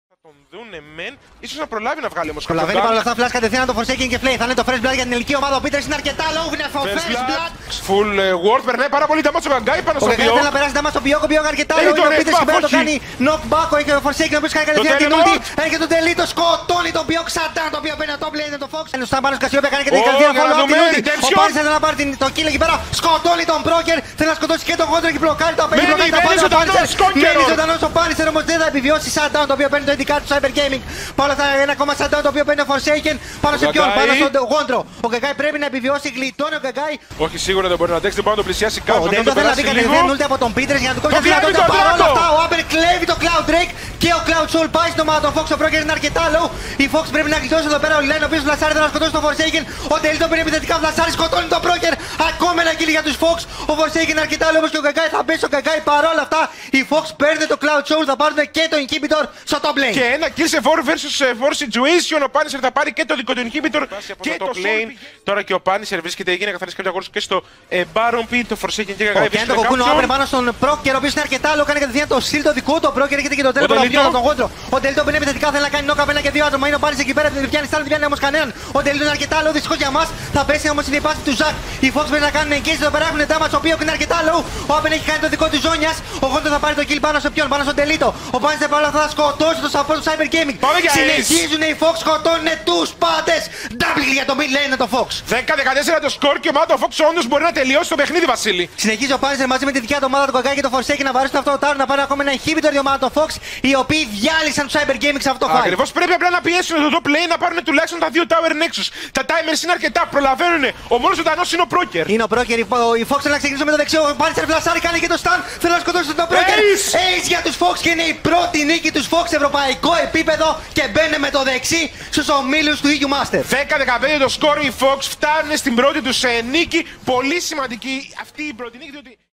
Highlights IF Forsaken with Thresh - EM 2020 Spring Play-In
Cyber Gaming vs Intrepid Fox - EuMasters 2020 Play-In